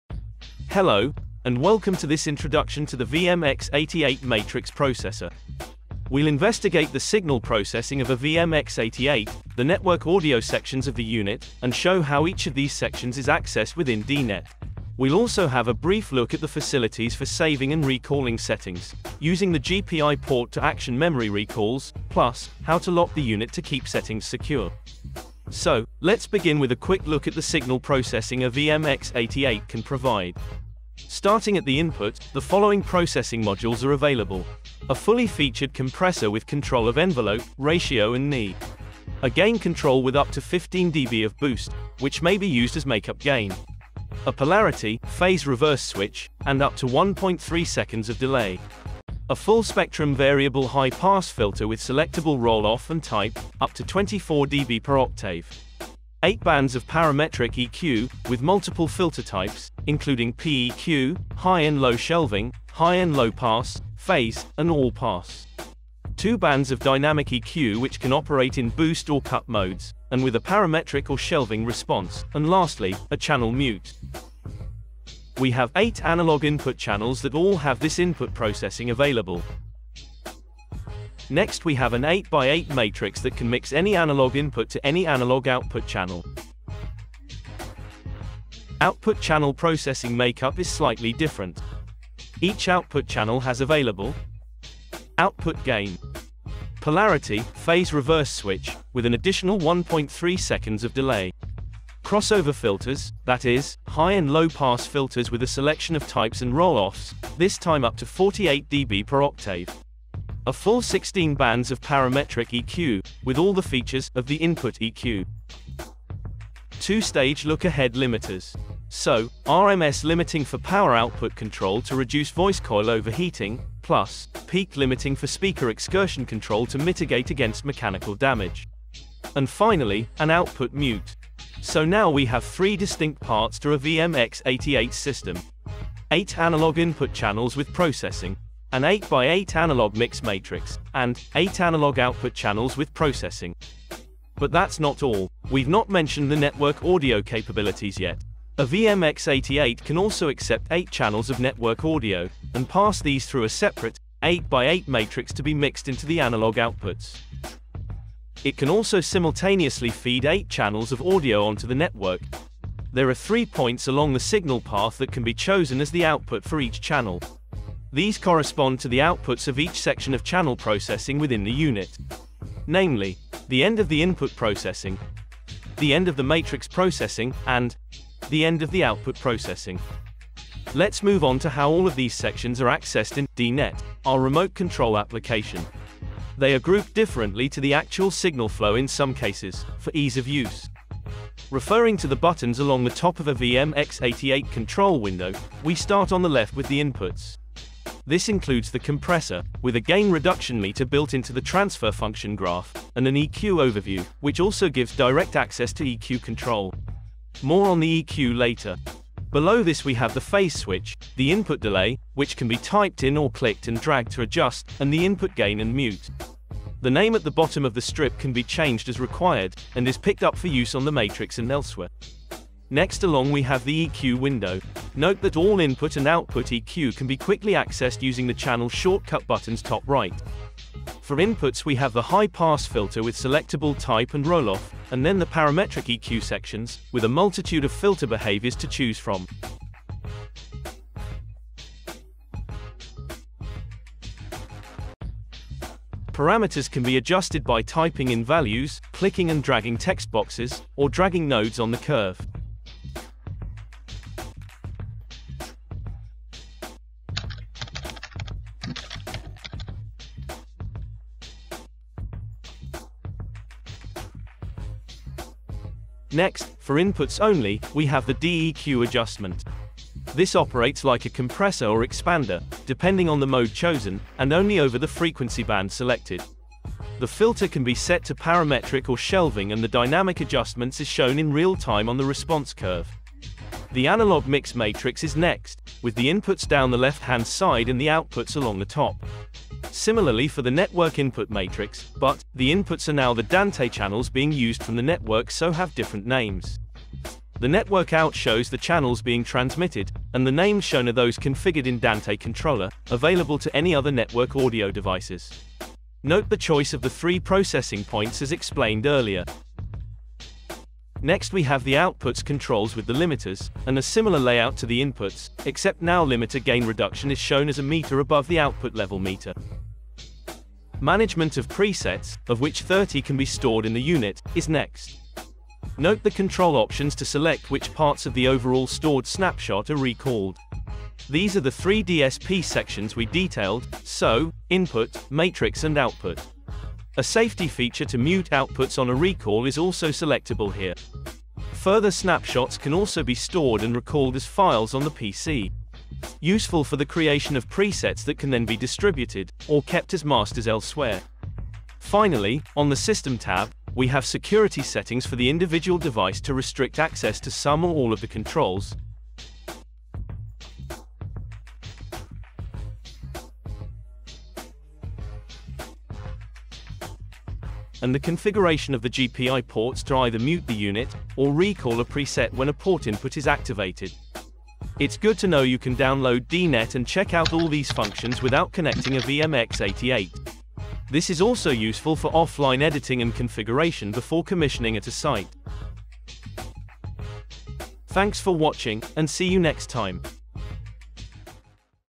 What's in a VMX88?
What's in a VMX88?  DSP, Network Audio, GPI, clever remote control...we explain it all!